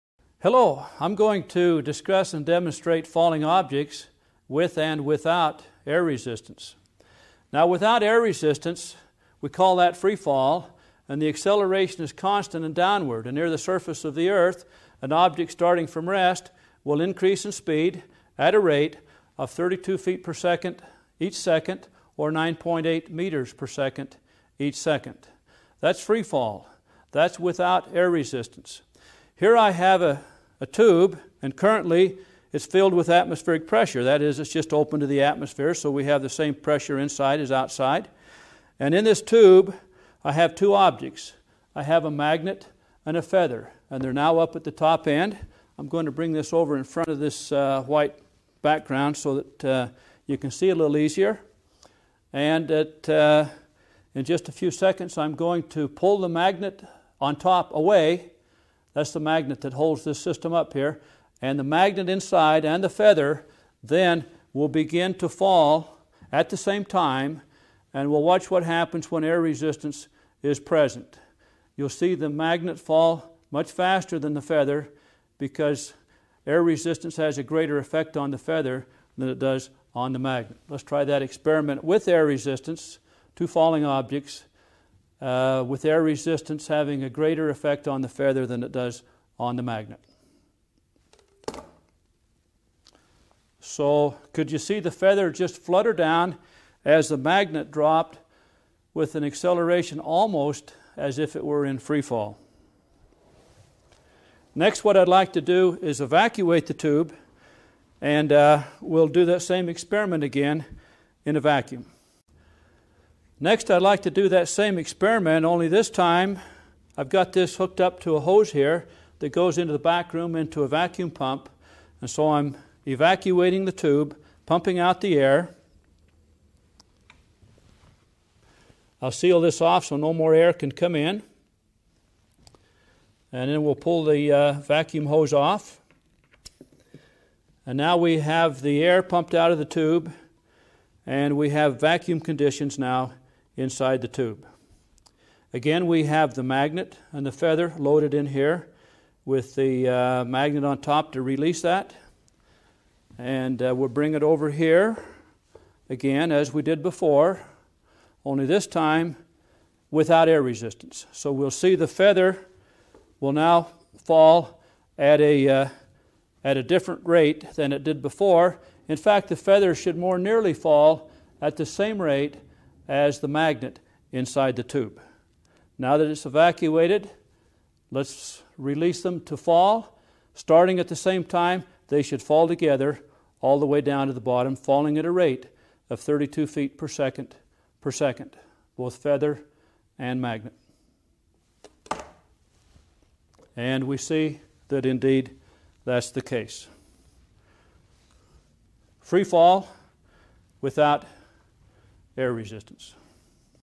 Feather and Coin
A feather and a coin are allowed to simultaneously begin falling from rest while inside a glass tube. In the first experiment, the tube is open to the atmosphere and the feather flutters down much more slowly than the coin. In the second experiment, the tube is evacuated of air and the feather and coin fall together.

Equipment:
- Glass tube-feather-coin system
- Vacuum pump and hose